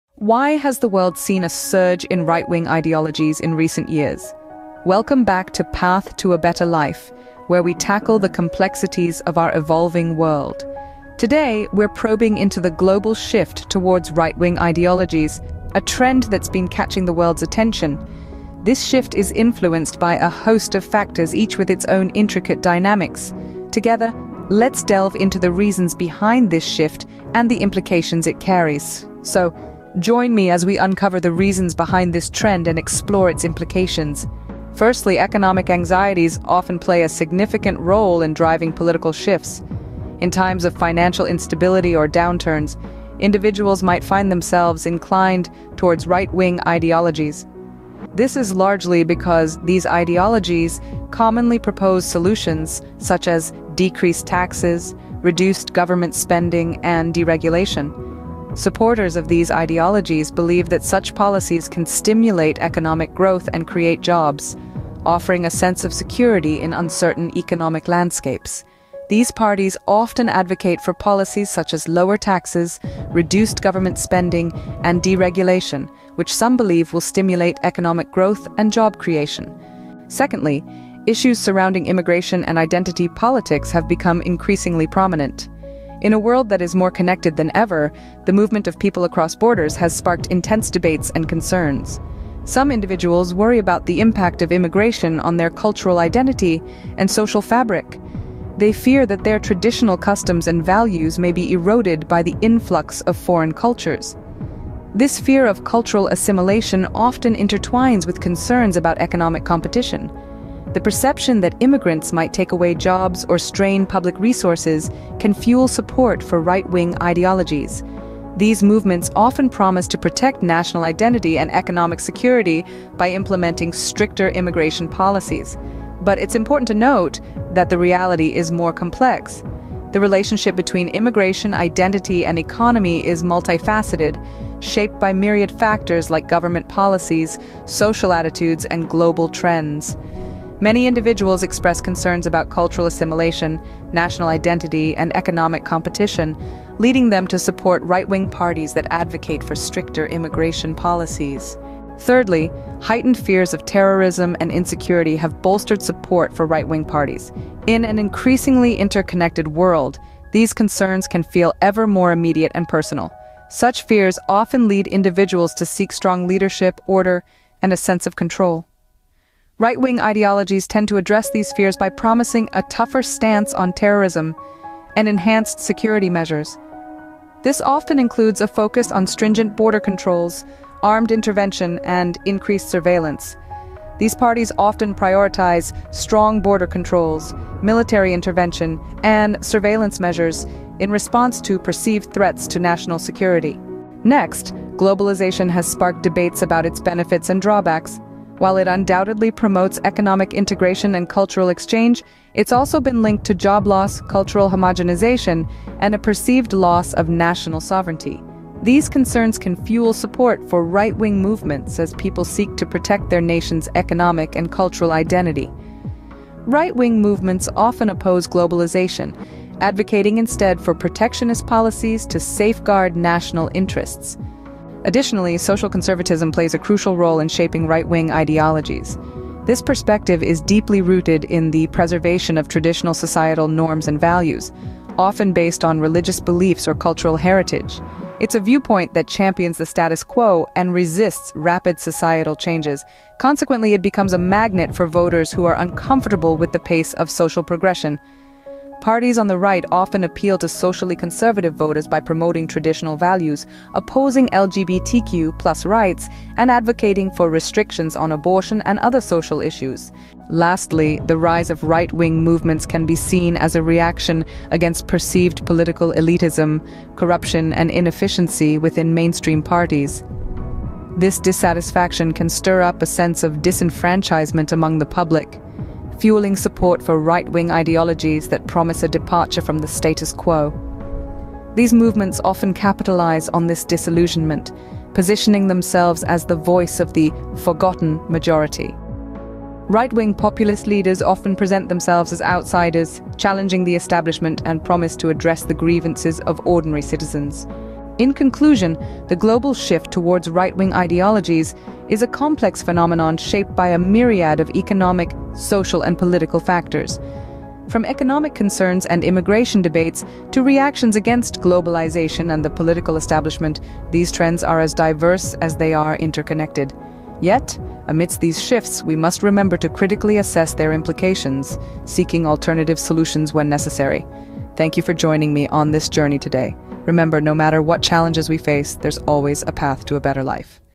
The Surge of Right-Wing Ideologies: Understanding the Shift
In this compelling YouTube video, we delve into the complex phenomenon behind the ascension of right-wing ideologies in contemporary societies. Through meticulous analysis and expert insights, viewers gain a nuanced understanding of the multifaceted factors contributing to this ideological shift.

The video begins by examining socio-economic anxieties prevalent in many Western nations, including concerns about globalization, immigration, and job insecurity. It explores how these anxieties intersect with cultural and identity-related issues, such as perceived threats to traditional values and national identity.

Moreover, the rise of social media and digital platforms has provided fertile ground for the dissemination of right-wing narratives, often characterized by polarization, misinformation, and echo chambers. The video elucidates how online communities and algorithmic biases reinforce ideological divides, fostering an environment conducive to the proliferation of extremist viewpoints.

Additionally, the failure of mainstream political establishments to effectively address these concerns has fueled disillusionment among segments of the population, paving the way for the rise of populist leaders and movements espousing right-wing ideologies.

Through incisive commentary and compelling visuals, this video offers viewers a comprehensive exploration of the complex dynamics driving the surge of right-wing ideologies, urging them to critically evaluate the societal forces shaping our political landscape.